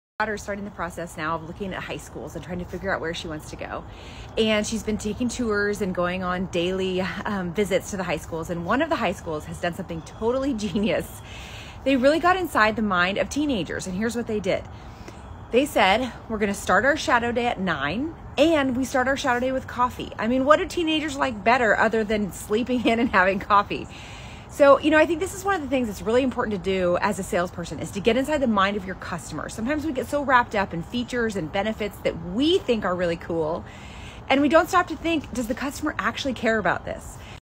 What Customers Really Want | Golden Rule of Selling with Katie Mullen
We've been touring high schools this month, and one school did something really genius.

They told us to be there at 9:00 (instead of 7:45), and they told us the day would start with coffee.

They sure got inside the mind of teenagers...I mean, what do teenagers like more than sleeping in and coffee?

Sometimes we get so wrapped up in cool features that WE think are cool, and we don't stop to think...would this particular customer ACTUALLY care about this?

If you don't know, ask them!